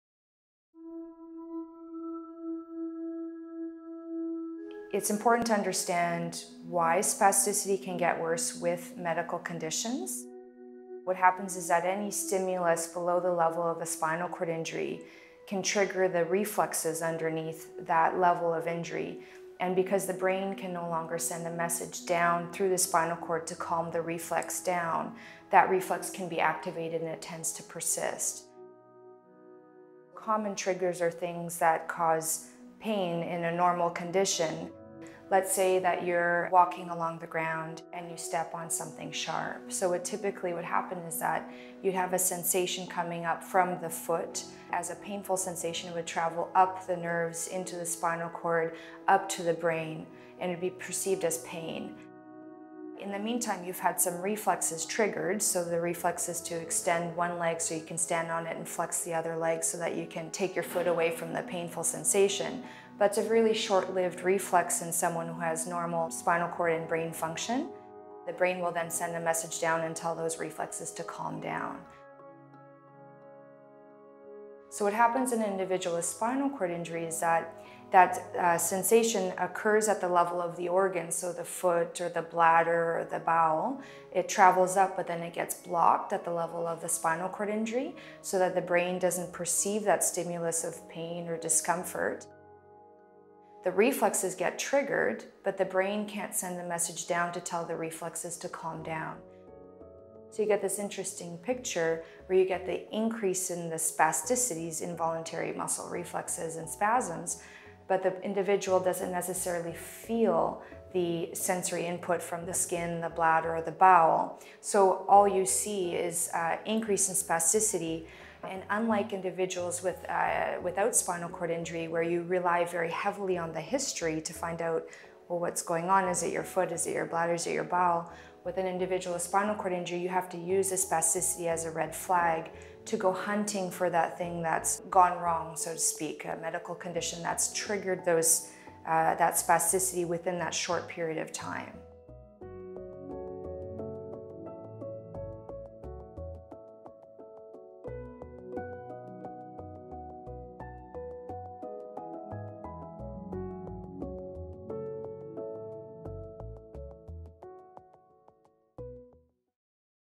Spasticity and Spinal Cord Injury: Physiological Basis
Up to 90% of people with spinal cord injury (SCI) have some form of spasticity - quick and involuntary muscle spasms. Because the brain can no longer send a message down through the spinal cord to calm the reflex down, that reflex can be activated and it tends to persist.